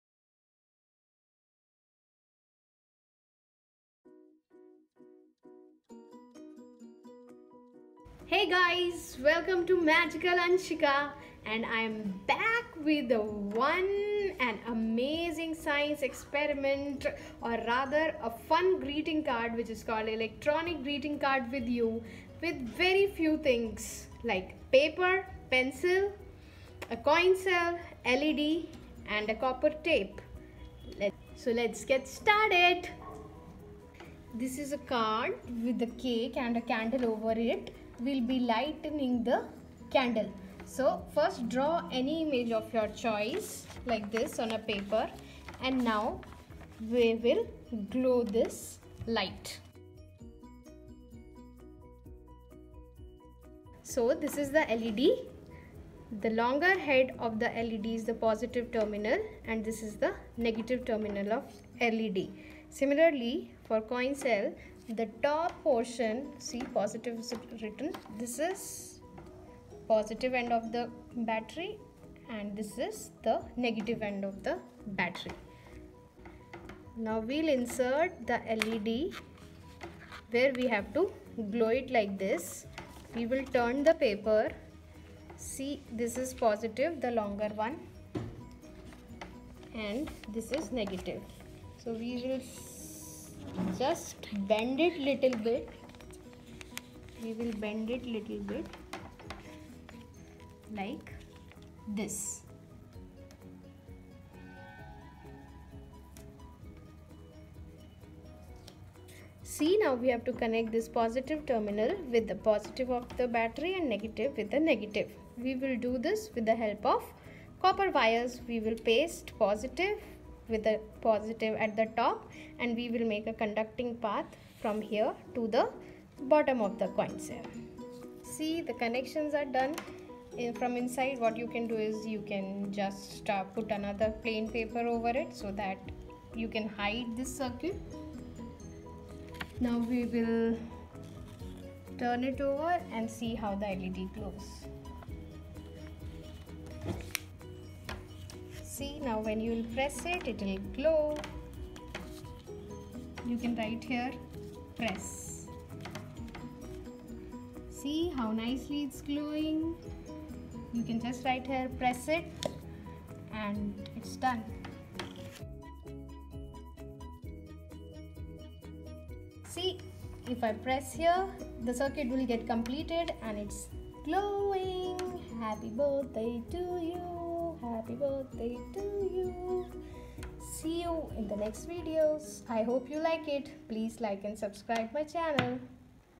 How to make electronic Greeting Card | Science Projects | Kids | Children | E-cards
In this video will help you to create electronic greeting cards. Kids will enjoy a lot.